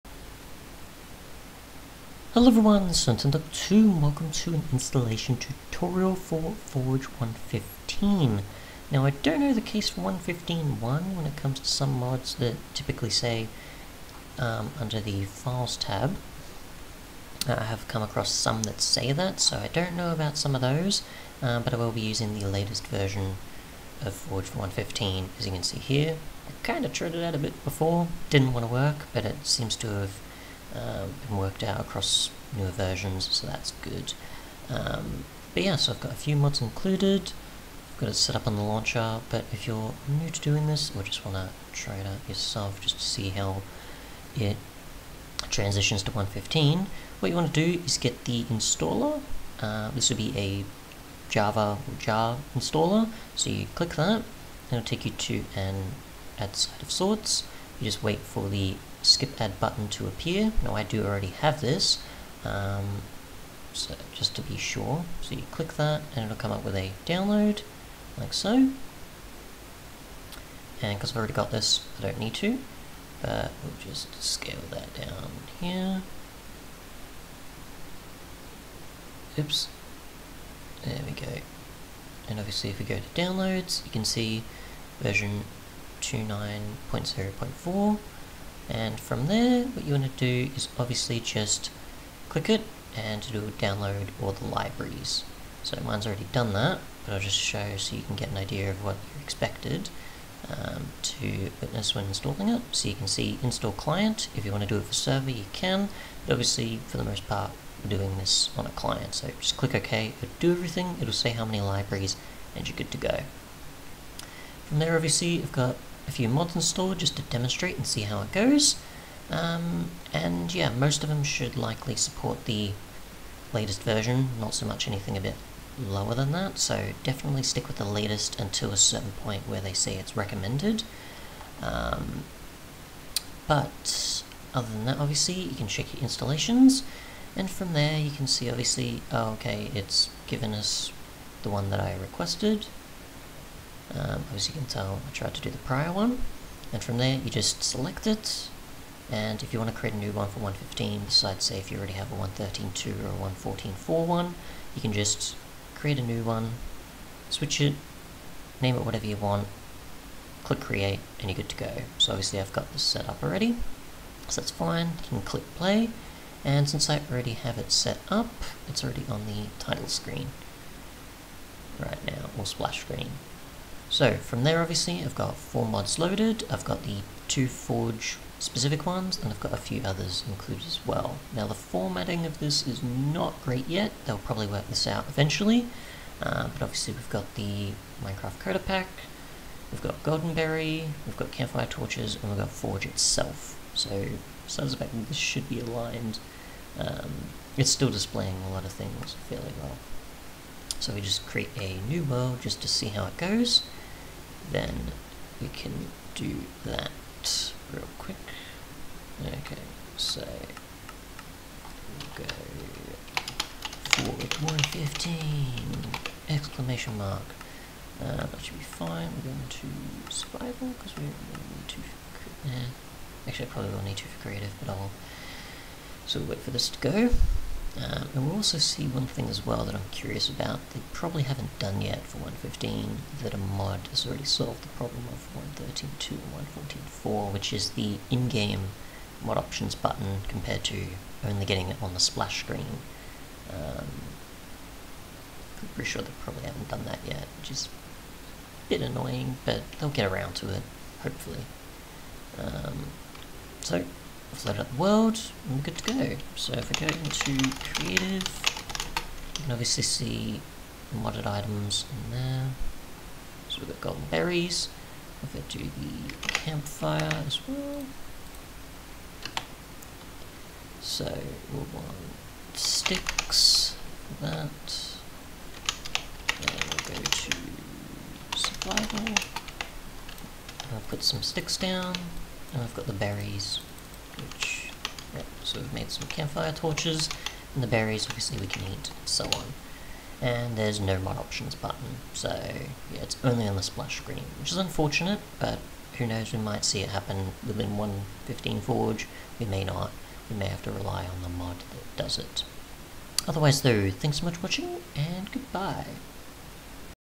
Forge 1.15-1.15.2 Vanilla Install Tutorial [works from 1.6.2+]
A tutorial showing how to install Forge for 1.15 into the Vanilla Launcher and showing how it worked out with mods installed. Installation for modding has not changed in years.

Mod Links:
Forge 1.15:

Other info:
Java 8 if needed, won't work for 1.17+

Mojang offers Java itself with the downloads of Vanilla but Modloader either take with that as come across or at least during 1.17 pre releases had to go around as far as remember.